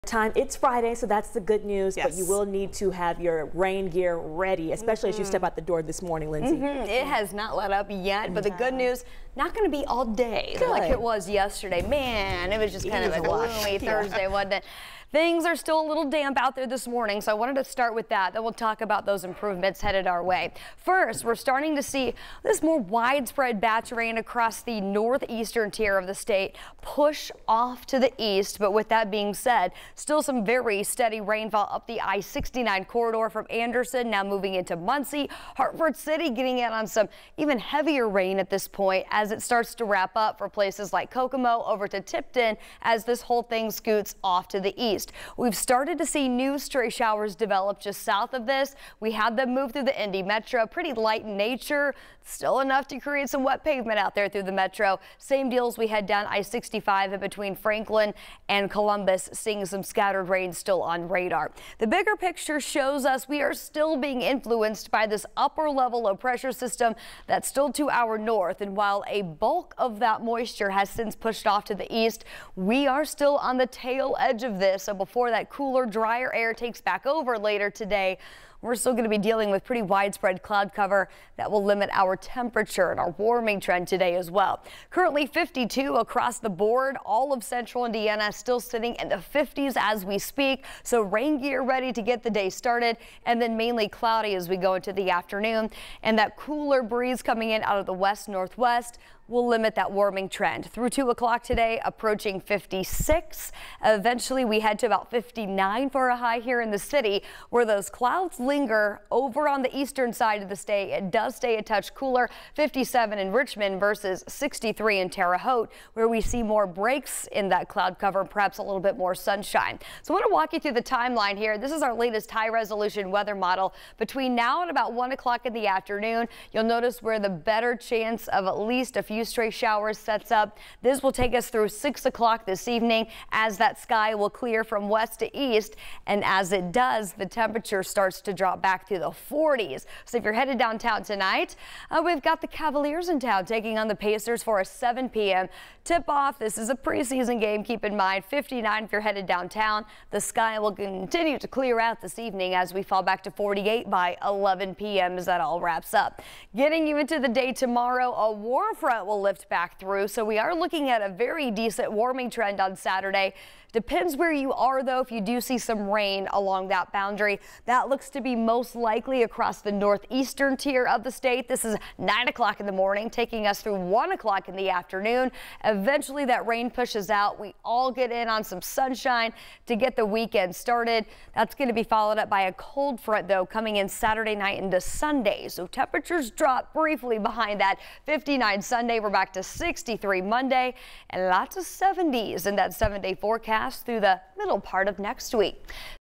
Live Doppler 13 morning forecast - Friday, Oct. 20, 2023
We're waking up to lingering rain showers across the state, but the better chance of showers will stay mainly east through the afternoon. Lindsey's morning forecast for Friday, Oct. 20, 2023.